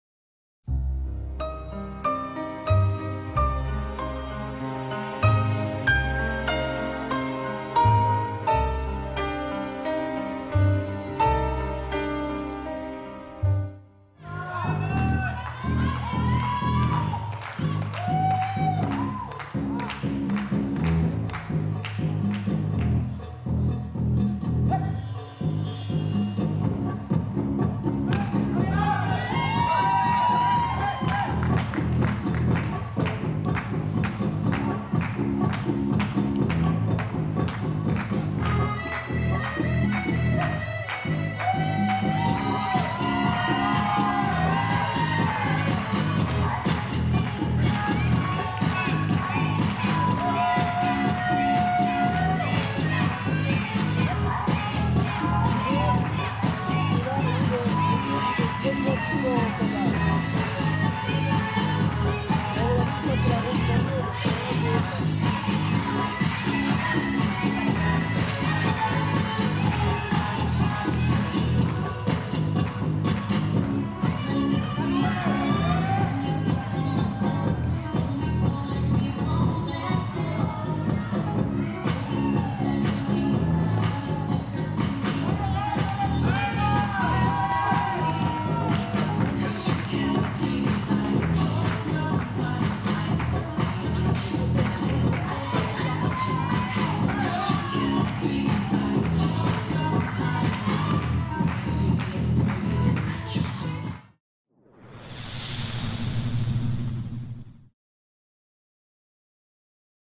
2013 World SEN I 10D | The Final Cha Cha Cha
The finalists dance the Cha Cha Cha in the 2013 World DanceSport Championship Senior I Ten Dance in Antwerp, BEL.